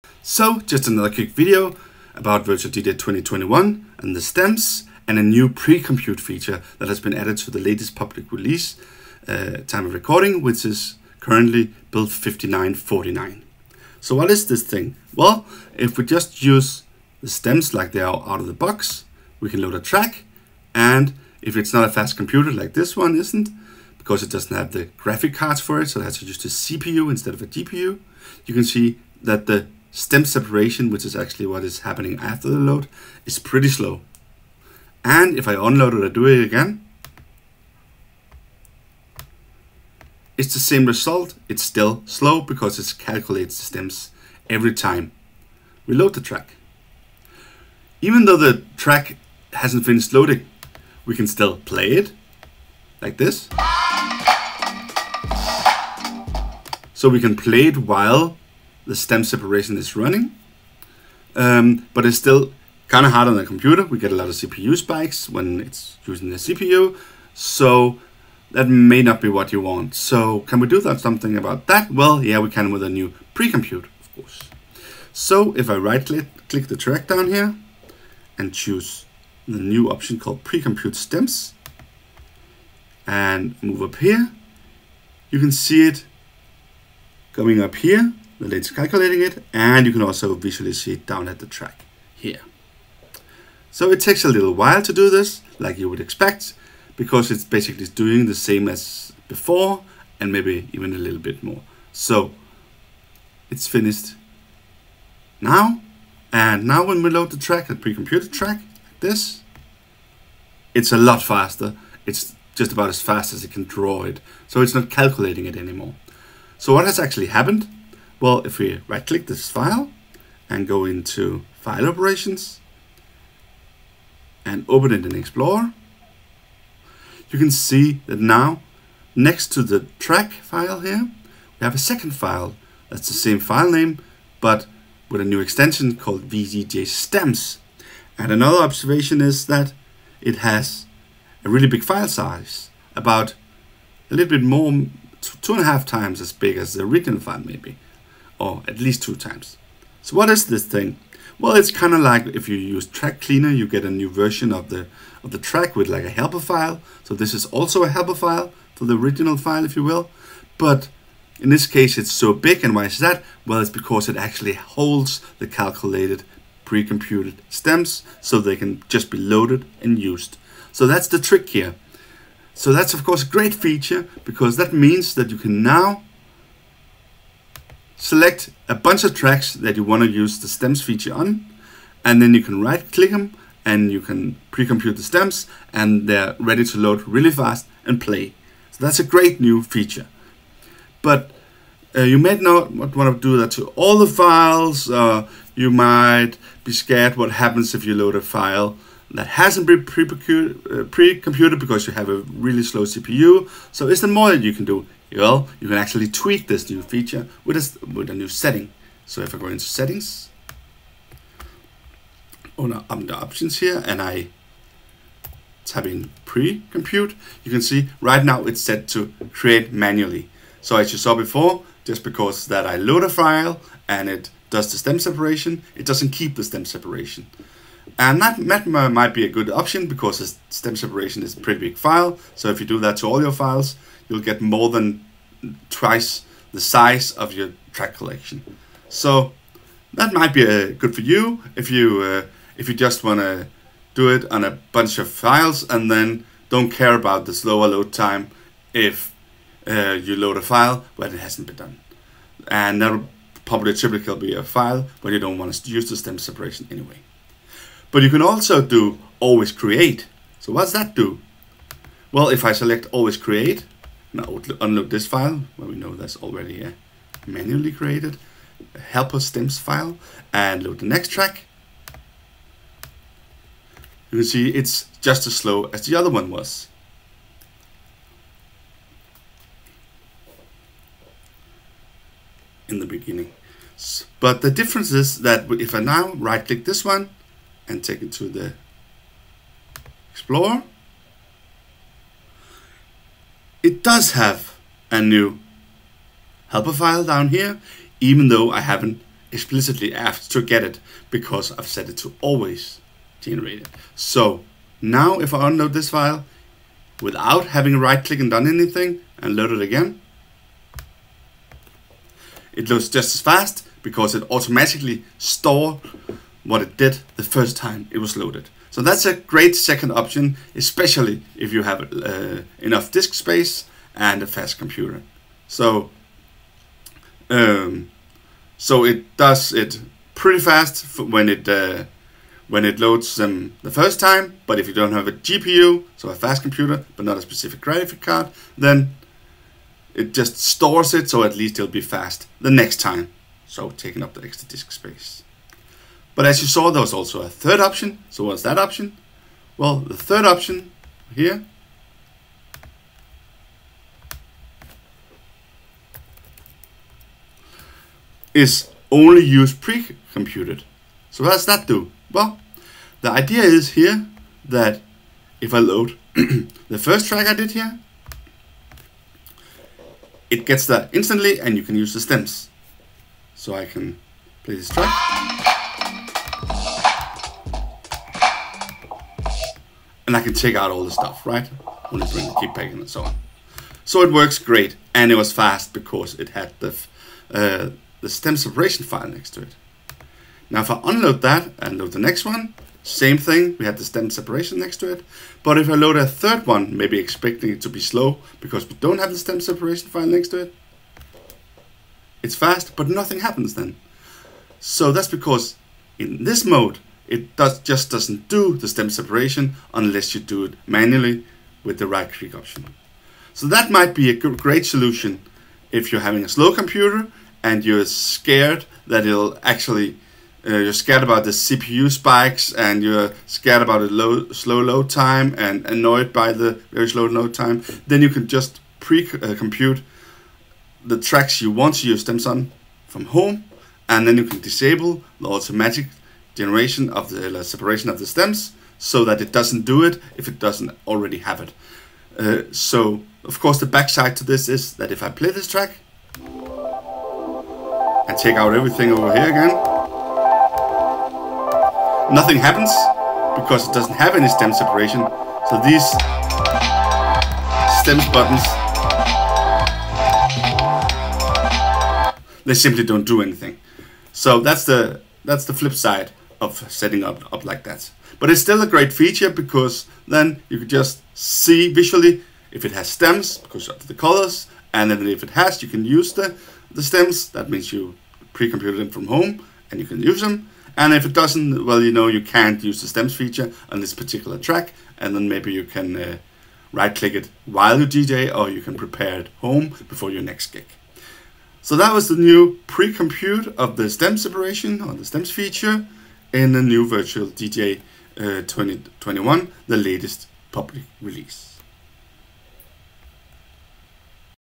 Quick n' Dirty: Pre-compute STEMS in VDJ8.5/2021 build 5949
This video shows how you can use the new pre-compute STEMS feature in VDJ8.5/2021 build 5949, and goes into detail about the 3 different pre-compute modes